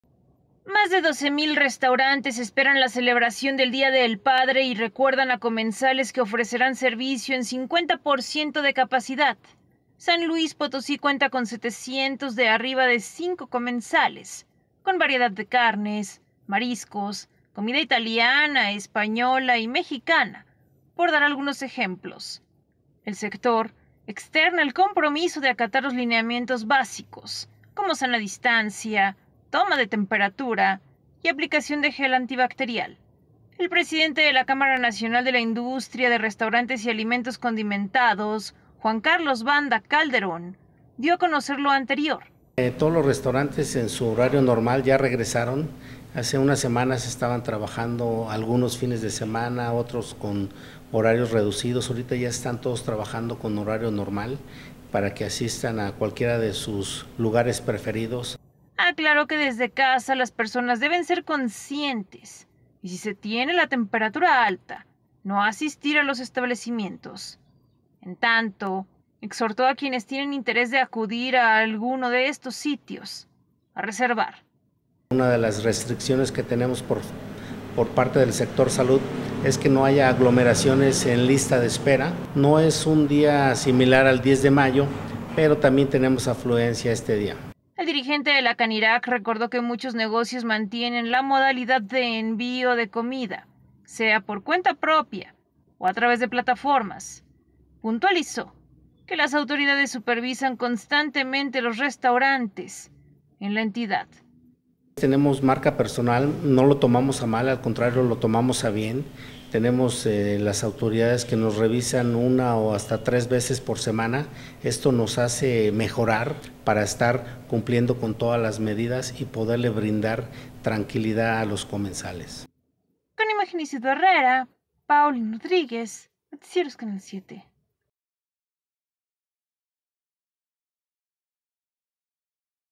Restaurantes en San Luis Potosí estarán abiertos este Día del Padre.
SLP.- Más de 12 mil restaurantes esperan la celebración del Día del Padre y recuerdan a comensales que ofrecerán servicio en 50 por ciento de capacidad.

San Luis Potosí cuenta con 700 de arriba de cinco comensales, con variedad de carnes, mariscos, comida italiana, española y mexicana -por dar algunos ejemplos-.

El sector externa el compromiso de acatar los lineamientos básicos de sanidad, como sana distancia, toma de temperatura y aplicación de gel antibacterial.

El presidente de la Cámara Nacional de la Industria de Restaurantes y Alimentos Condimentados (CANIRAC) Juan Carlos Banda Calderón dio a conocer lo anterior.

Aclaró que desde casa las personas deben ser conscientes y si se tiene la temperatura alta, no asistir a los establecimientos.

En tanto, exhortó a quienes tienen interés de acudir a alguno de estos sitios, a reservar.

El dirigente de la CANIRAC recordó que muchos negocios mantienen la modalidad de envío de comida, sea por cuenta propia o a través de plataformas.

Puntualizó que las autoridades supervisan constantemente los restaurantes en la entidad.